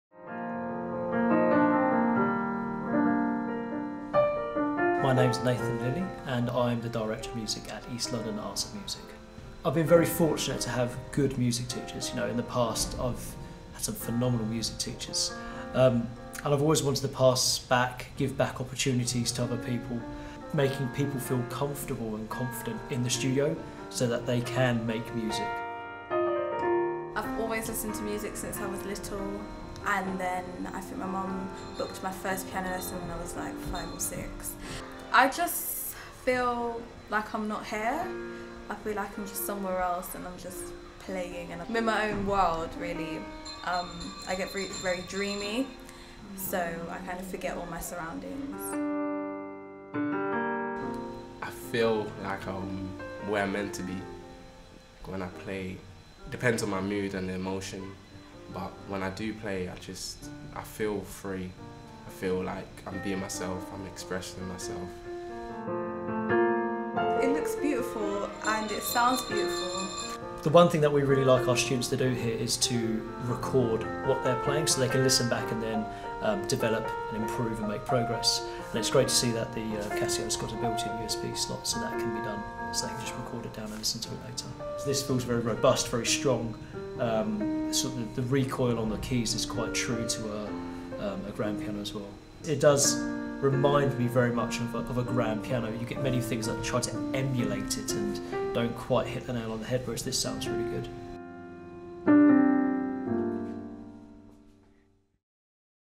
ELAM (East London Arts & Music) join the Action in Music
The latest edition of our Action in Music initiative saw us delve into the heart of one of London’s most modern free schools dedicated to the Arts and Music, as we continue to celebrate the inspiring work of Music Teacher's UK Wide.